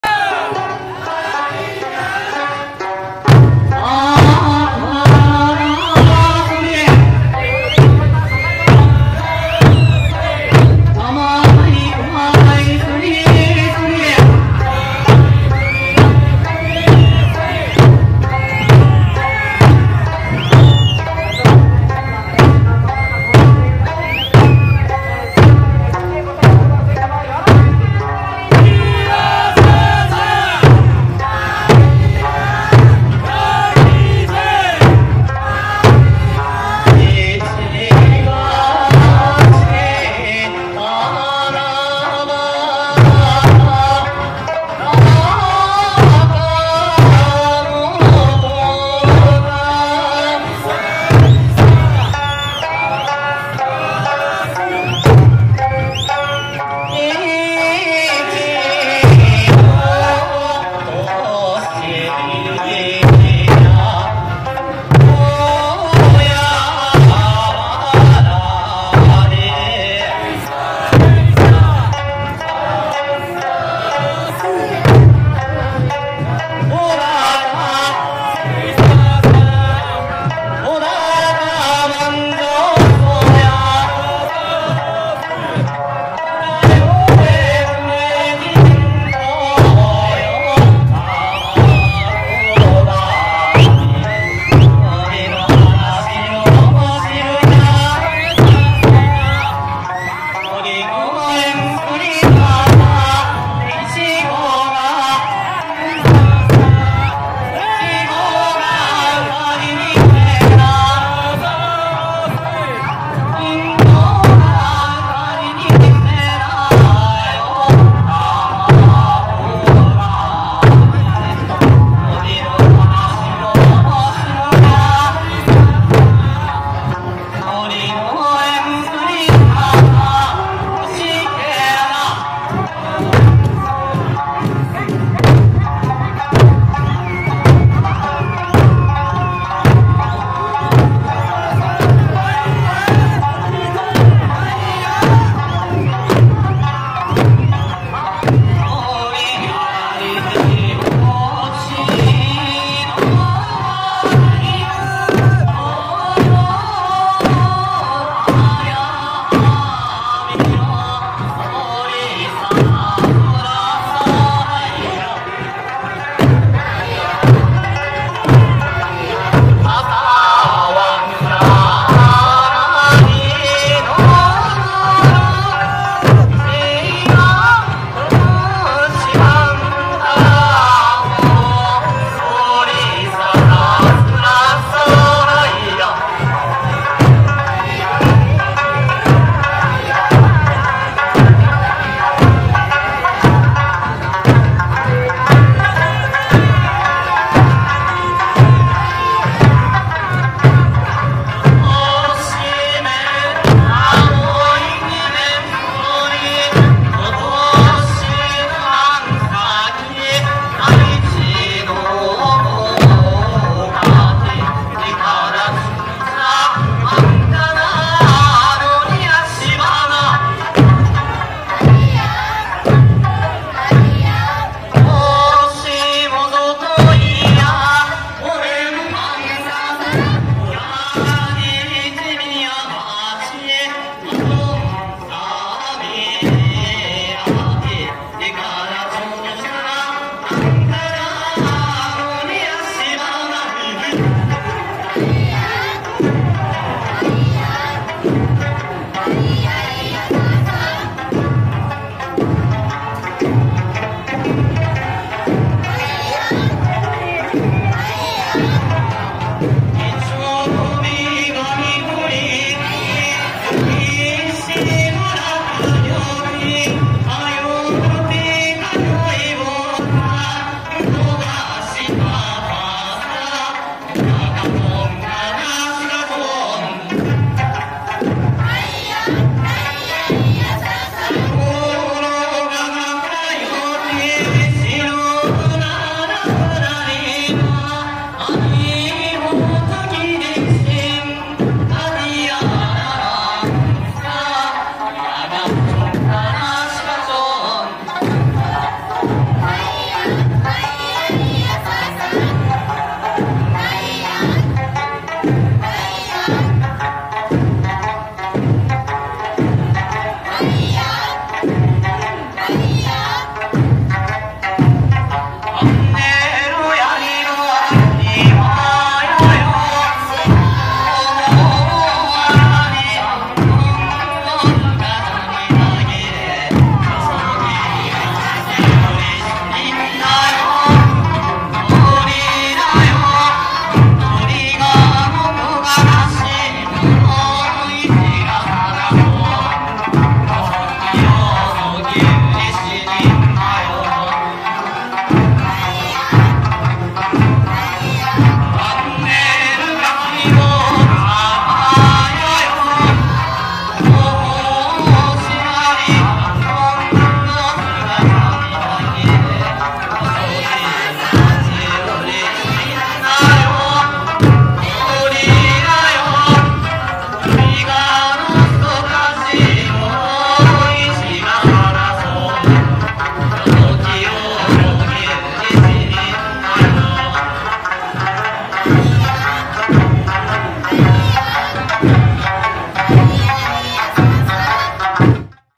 渡慶次青年会 エイサー天国「読谷村高志保大通り」2024.8.11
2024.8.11　「第28回読谷村高志保大通りエイサー天国」にて行われた「読谷村渡慶次青年会」によるエイサーの演舞です。渡慶次青年会